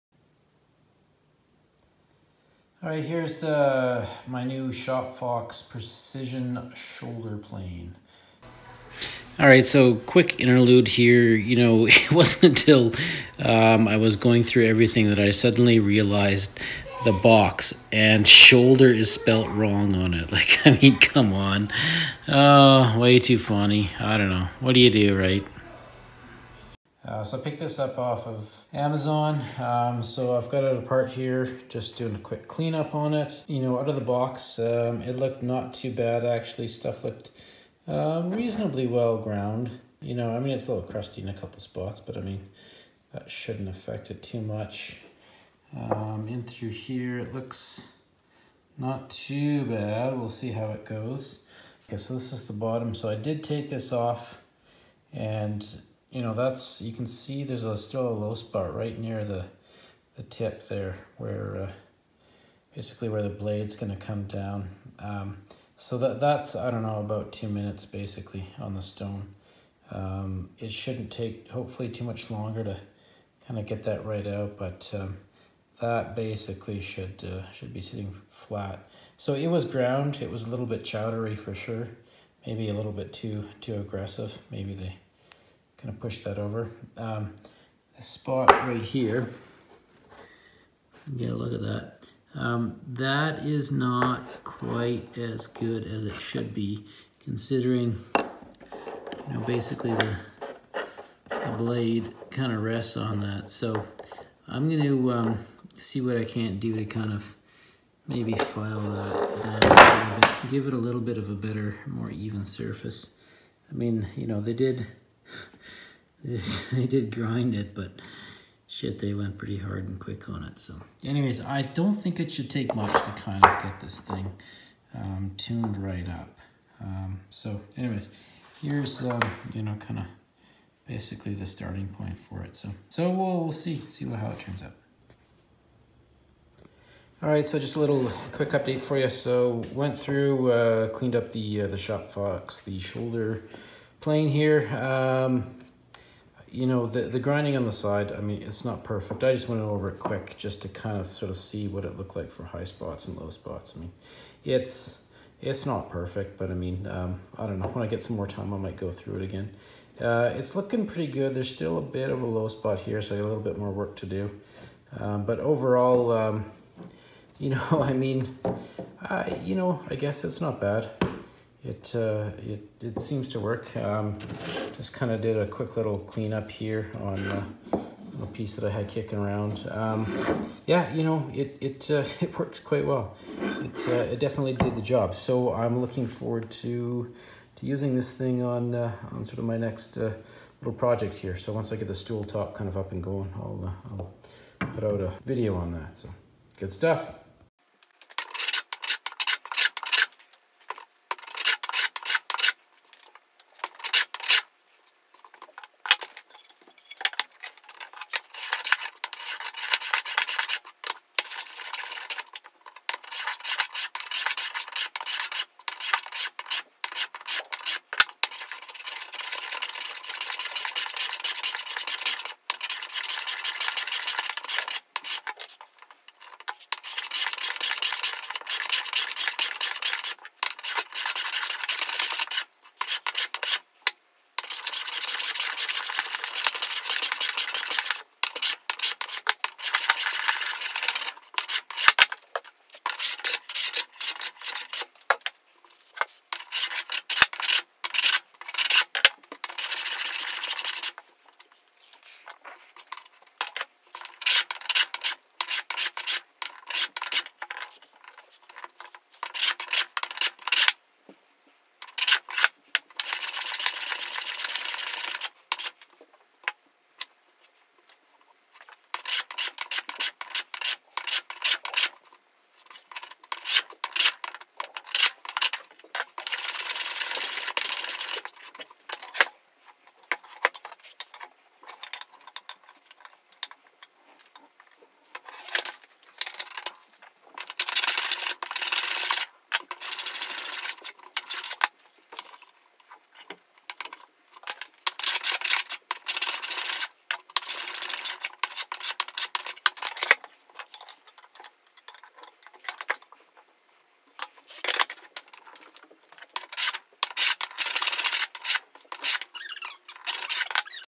Shop Fox Precision Shoulder Plane D3751
Quick review of the Shop Fox D3751 Precision Shoulder Plane